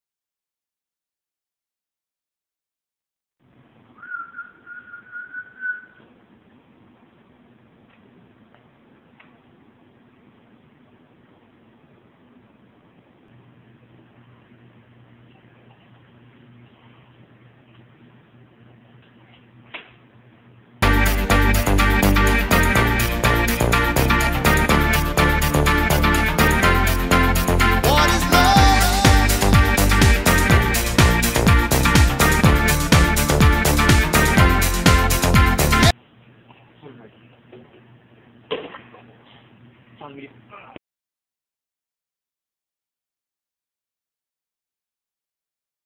Coke JukeBox
mike and will at a coke machine